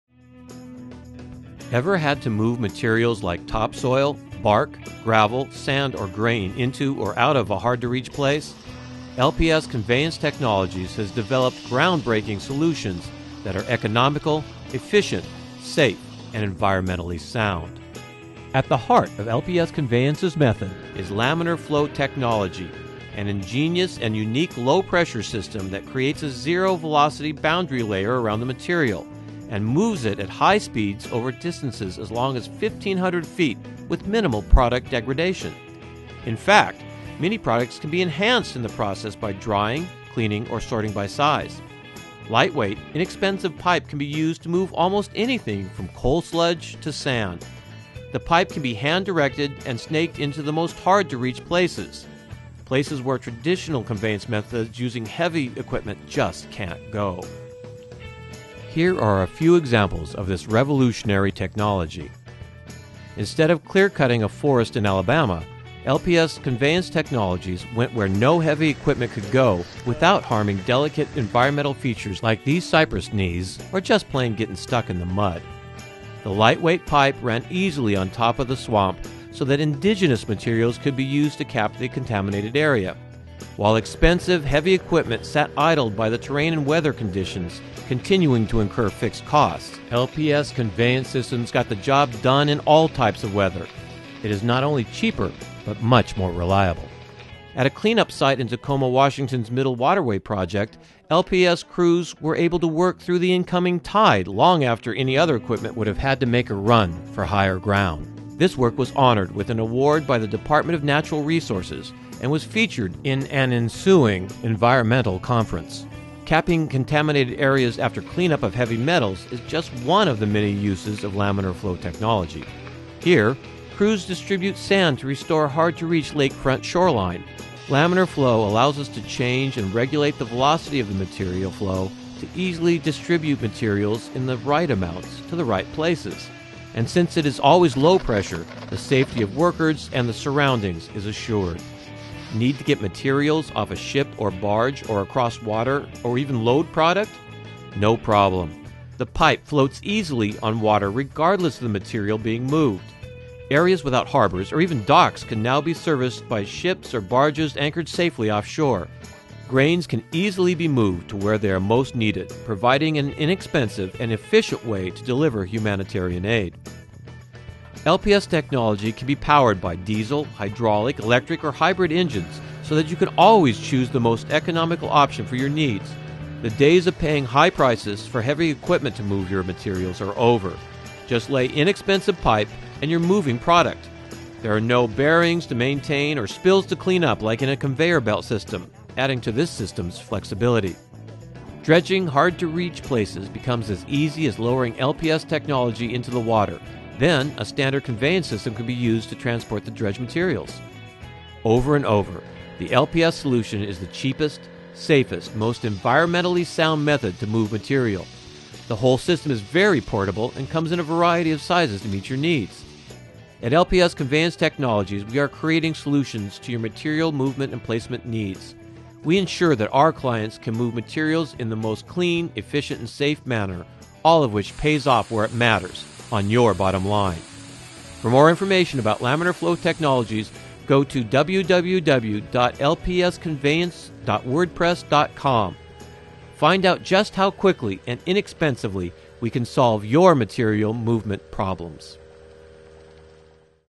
LPS Conveyance - Moving Bulk Materials with Air
Breakthrough technology moves bulk material through flexible pipe on a cushion of air.  Must be seen to be believed.